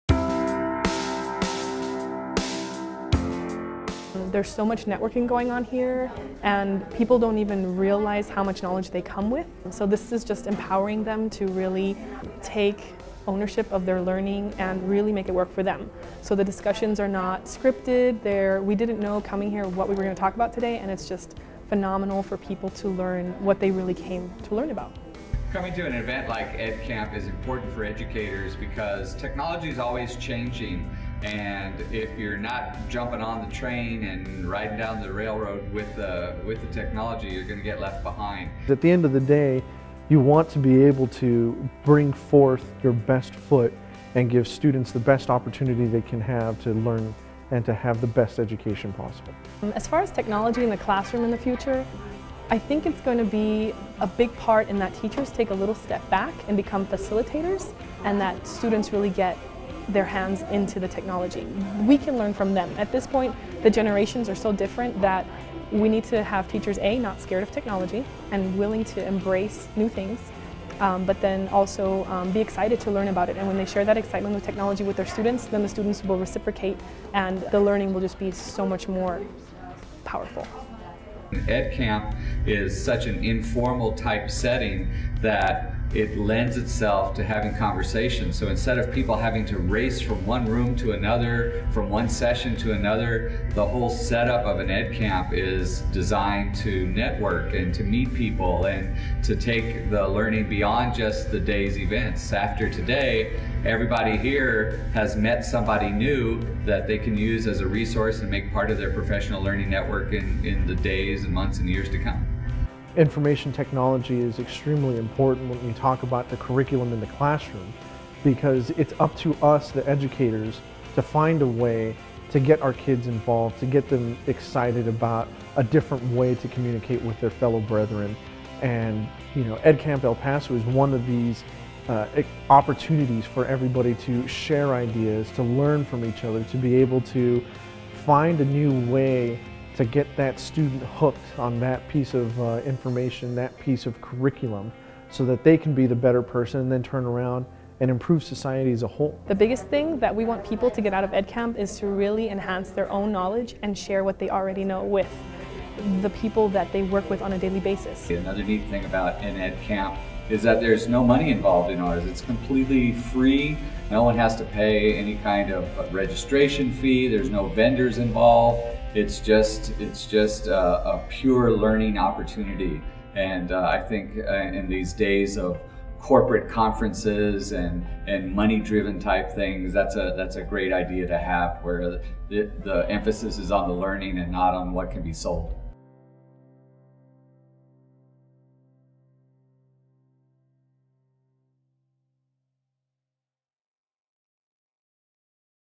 ED Camp El Paso
What is an Ed Camp? Here is a short video the Ed Camp El Paso folks created to encourage you to make your own EdCamp!